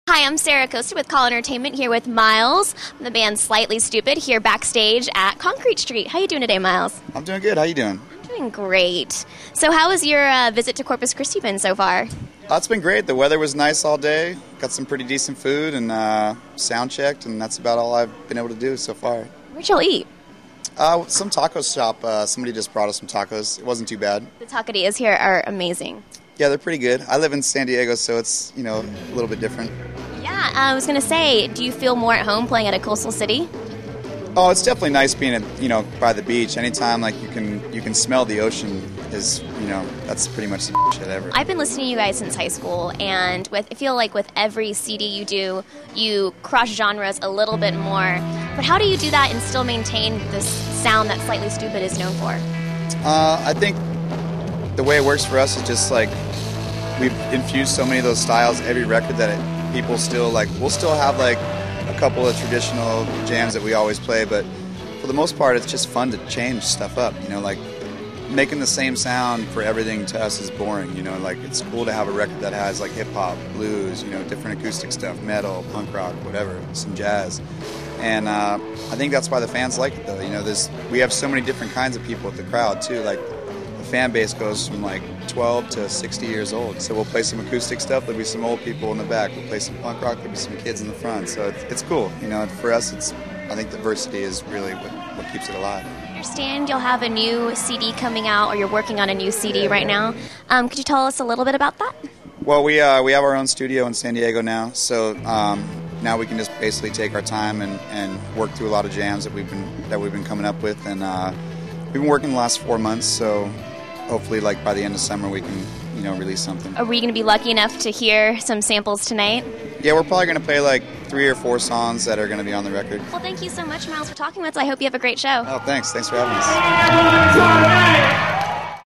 Caller.com Entertainment: Backstage with Slightly Stoopid
Watch Caller Entertainment Reporter Sarah Acosta chat with Miles Doughty, the lead singer of the reggae/rock band Slightly Stoopid, before the band's show at Concrete Street.